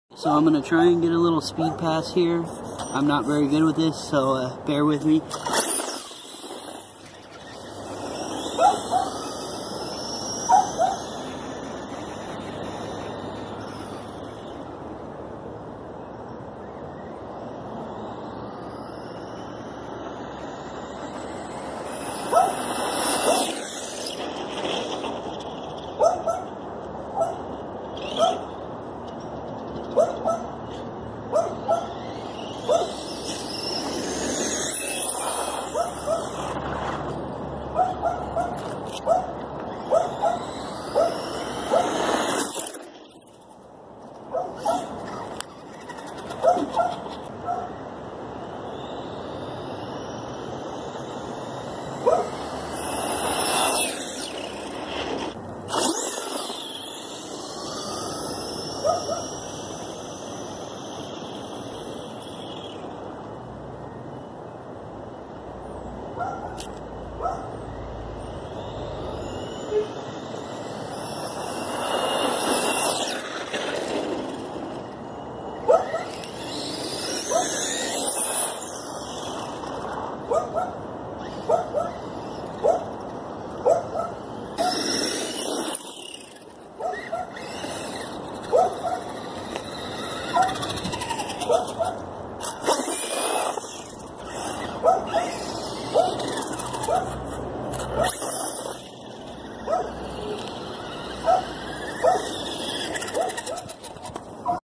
5900kv 2wd stampede speed pass!
Here a short video of a few speed pass's I did with my 5900kv castle powered stampede! Sorry camera don't do it justice and I don't had a GPS to put on it lol.
I will be taking the stampede out with my buddy's ruatlers soon I hope to! 😉